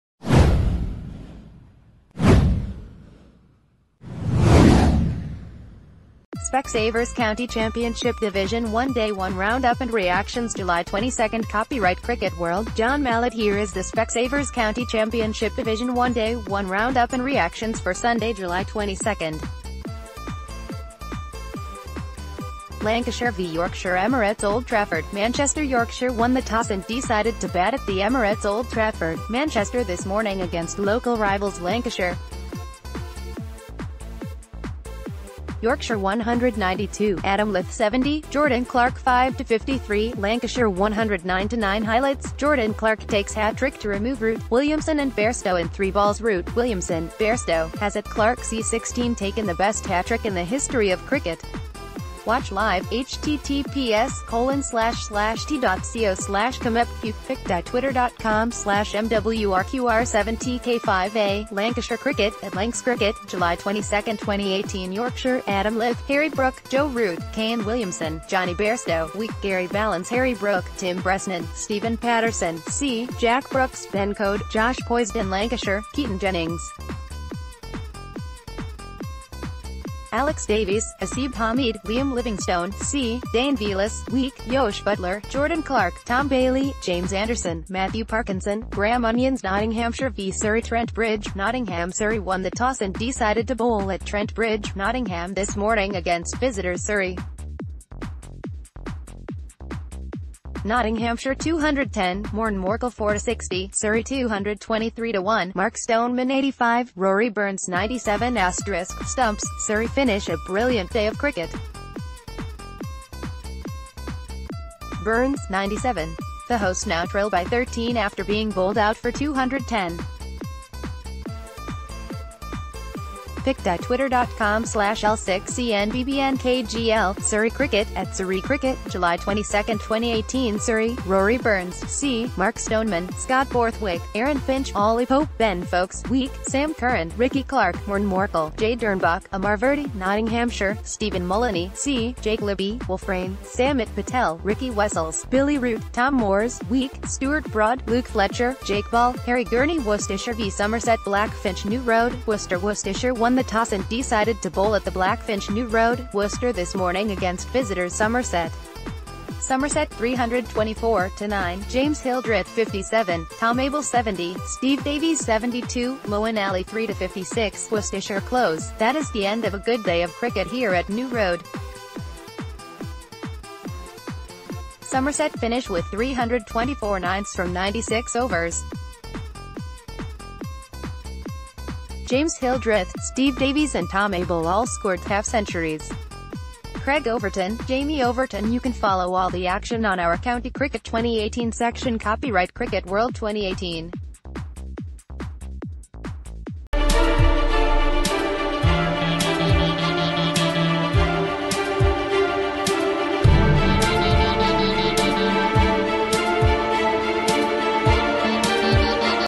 SpecSavers County Championship Division 1 Day 1 Round Up and Reactions July 22nd
SpecSavers County Championship Division 1 Day 1 Round Up and Reactions July 22nd ©Cricket World / John Mallett Here is the SpecSavers County Championship Division 1 Day 1 Round Up and Reactions for Sunday July 22nd. Lancashire v Yorkshire Emirates Old Trafford, Manchester Yorkshire won the toss and decided to bat at the Emirates Old Trafford, Manchester this morning against local rivals Lancashire. Yorkshire 192 (Adam Lyth 70;  Jordan Clark 5-53) Lancashire 109-9 Highlights – Jordan Clark takes ...